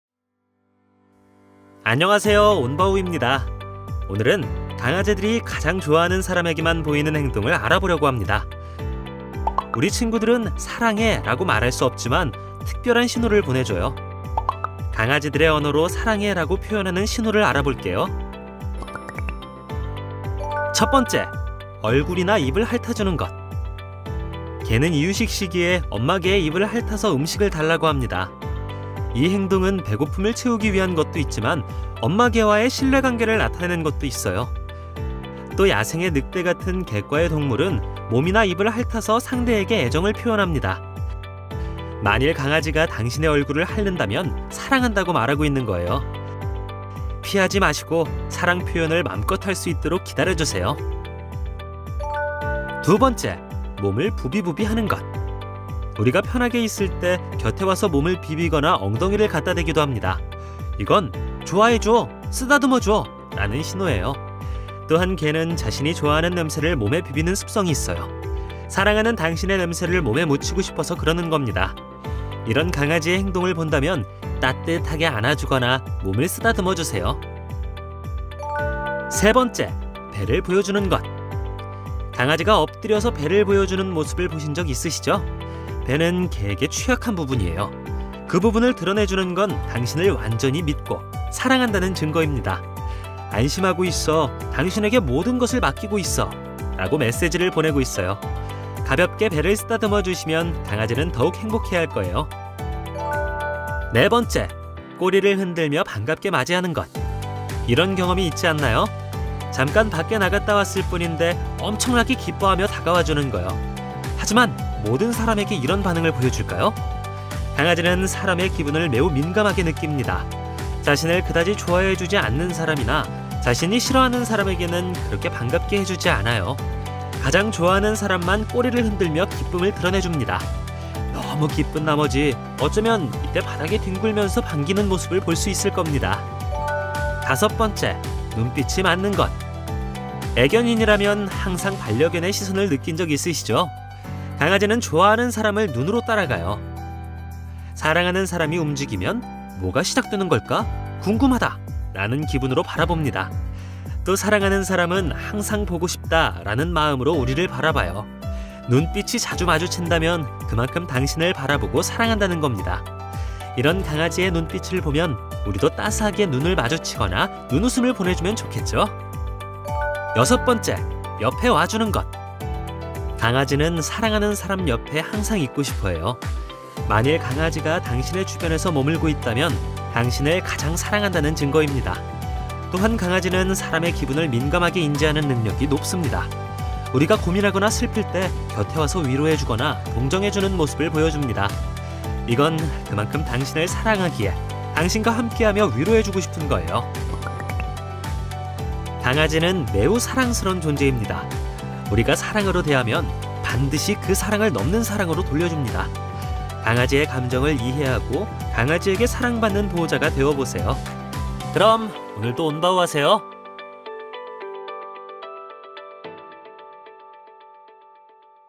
강아지가 당신을 사랑한다는 증거 6가지
우리 아이들은 사랑한다는 표현을 아주 적극적으로 합니다.
보호자가 모를 뿐이죠
오늘은 온바우와 함께 우리 아이들이 사랑하는 사람에게 하는
여섯가지를 행동을 알아보겠습니다

00:00 온바우
00:20 얼굴이나 입을 핥아주기
00:52 몸을 부비부비 하는 것
01:18 배를 보여주기
01:42 꼬리를 흔들며 반갑게 맞이하는 것
02:16 눈빛이 맞는 것
02:50 옆에 와주는 것